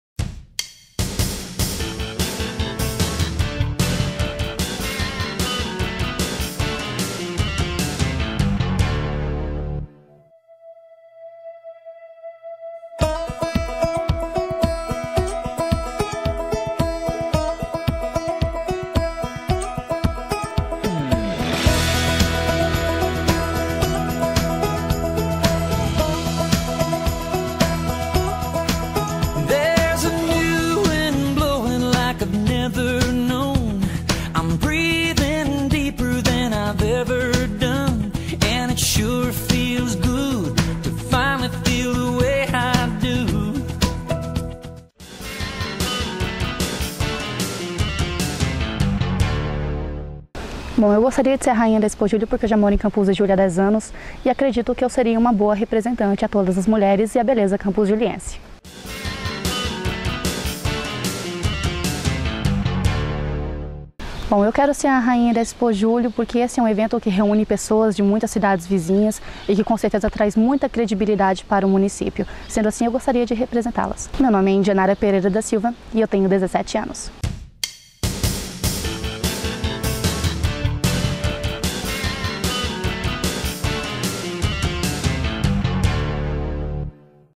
Indianara Pereira
Foram selecionadas as 10 finalistas que disputarão o título de Rainha e Princesas da EXPOJÚLIO 2012..